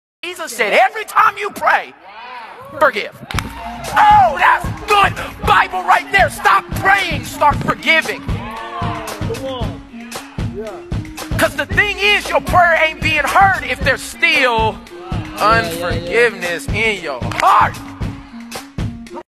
Victoria Adeyinka duet with @iammiketodd ‼️‼️‼️forgive and let your prayers be heard! God loves yo
duet with @iammiketodd ‼️‼️‼️forgive and let your prayers be heard! God loves you!💞